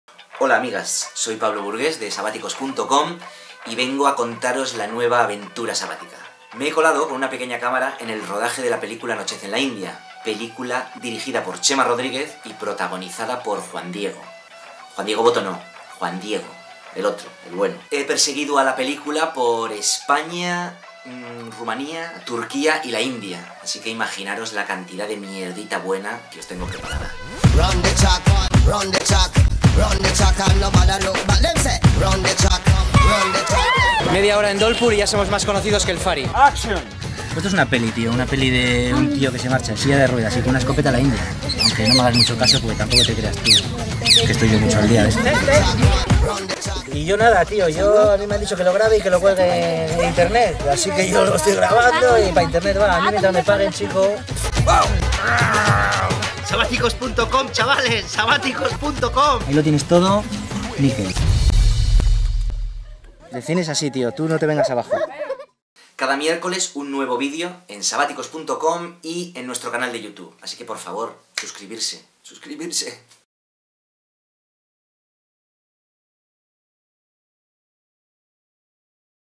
La aventura cinematográfica de Los Sabáticos
Pablo Burgués (de sabaticos.com) se ha colado con su cámara en el rodaje de la película "Anochece en la India", dirigida por Chema Rodríguez y protagonizada por Juan Diego.

Cada miércoles en sabaticos.com podremos seguir las mil y una freak-aventuras que sucedieron tras las cámaras de esta película grabada entre India, Turquía, Rumanía y España.